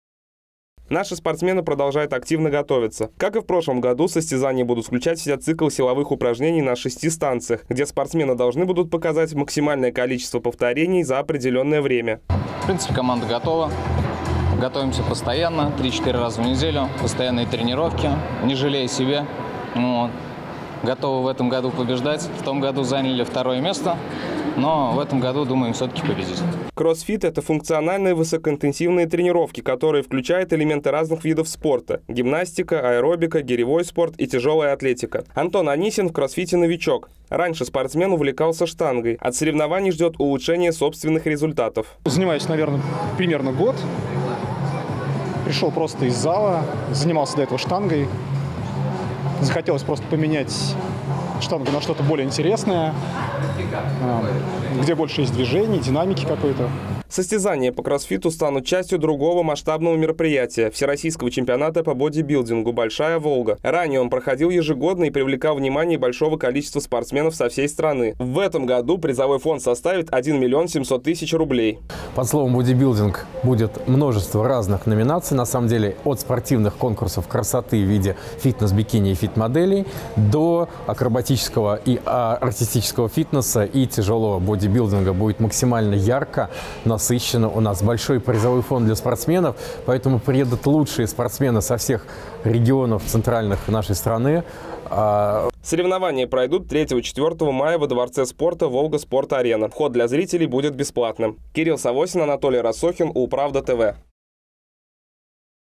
Ульяновские спортсмены готовятся к турниру по кроссфиту
С 3 по 4 мая кроссфитеры со всей страны соберутся в Ульяновске на турнире FITNESS CHALLENGE. Соревнования пройдут во второй раз, в рамках всероссийского чемпионата по бодибилдингу «Большая Волга». Как готовятся ульяновские спортсмены – узнавал Кирилл Савосин.